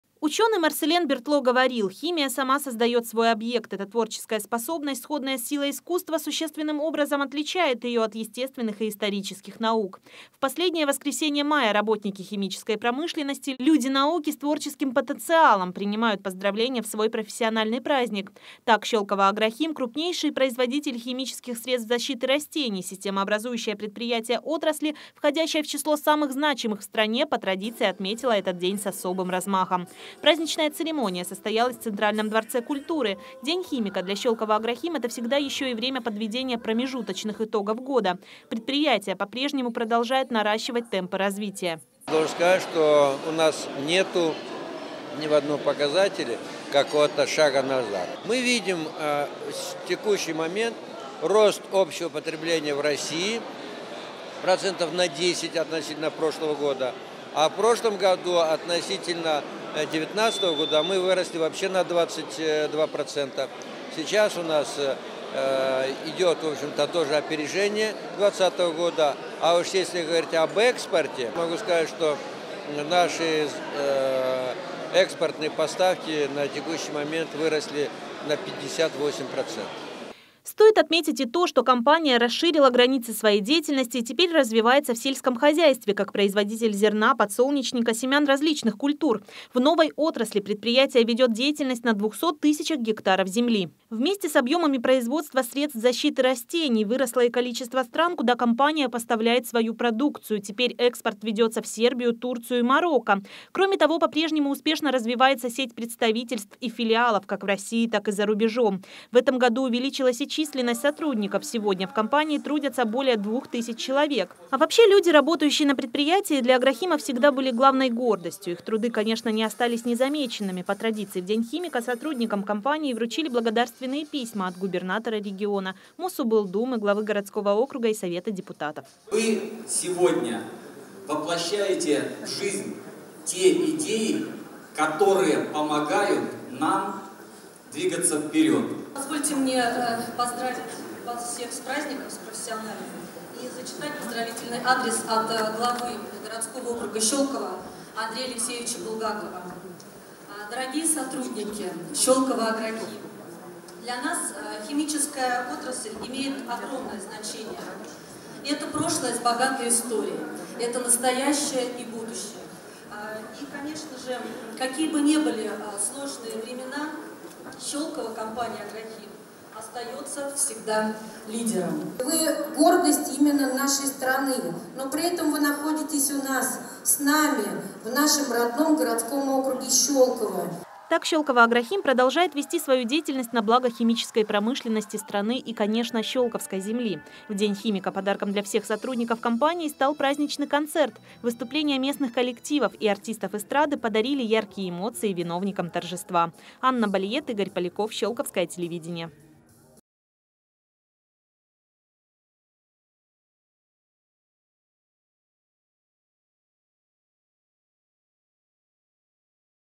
Компания «Щёлково Агрохим» отметила День химика
Профессиональный праздник отмечают работники химической промышленности. Ведущее предприятие в этой отрасли – «Щёлково Агрохим» встретило этот день в Центральном дворце культуры, где для сотрудников компании была организована концертная программа. Подробности у Анны Балиет.